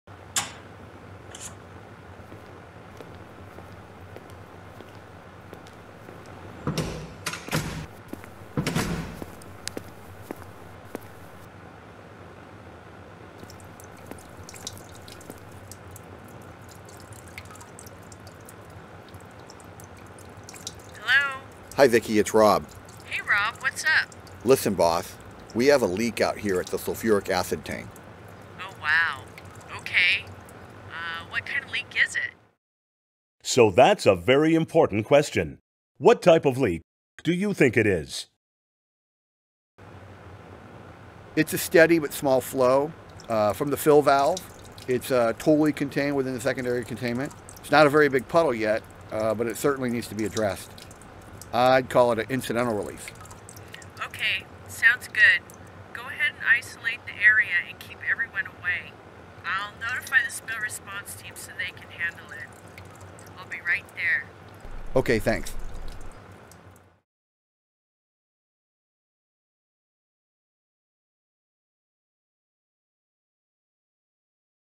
Hazmat Incidental Release Animation
Incidental Release

Rob finds a chemical leak from the sulfuric acid tank. He calls his boss (Vicky) to inform her of the leak.

The leak is a small, steady flow leaking from the fill valve and contained within the secondary containment, so Rob describes it as an incidental release.

Understanding how to describe a chemical leak becomes very important when making notifications. This effective communication will help tremendously in the Hazmat response.

Animation by Safety Unlimited, Inc.